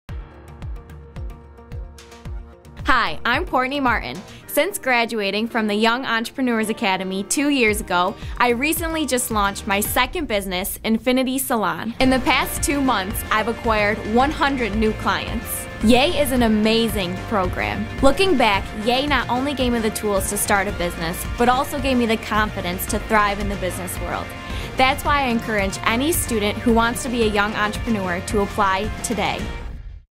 Nationwide YEA! TV Ad
Meet Courtney Martin, YEA! University of Rochester 2013 alum, and CEO of Infiniti Salon. Courtney learned how to start and run a business with the help of the YEA! program, giving her all of the tools she needed, every step of the way.

Do you know a middle or high school student with a BIG idea? Apply to the Young Entrepreneurs Academy today!